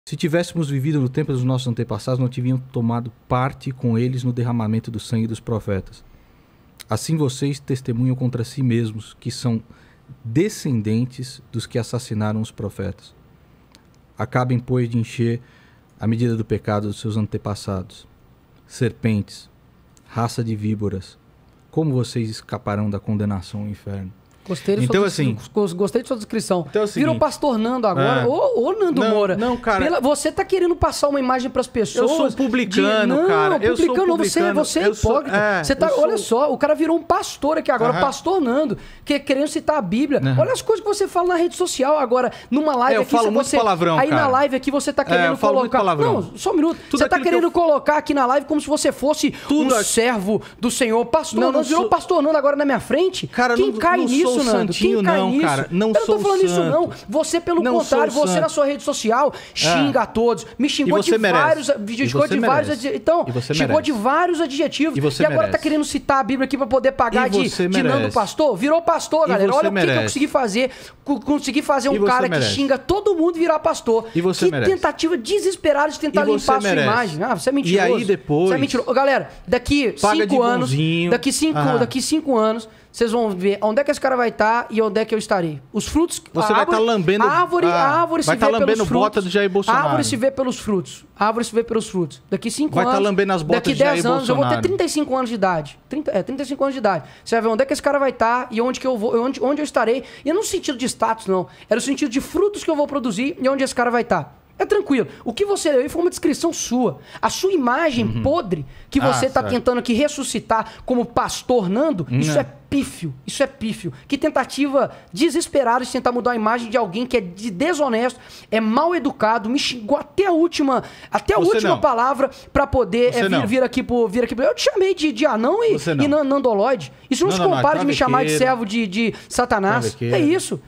NIKOLAS FOI CAPAZ ATÉ DE EXORCIZAR NANDO MOURA E CONVERTÊ-LO AO VIVO NO PASTOR DO PAU OCO
Nikolas foi capaz até de exorcizar Nando Moura e convertê-lo ao vivo no pastor do pau oco. Ou seria pastor do coco oco? Que humilhação!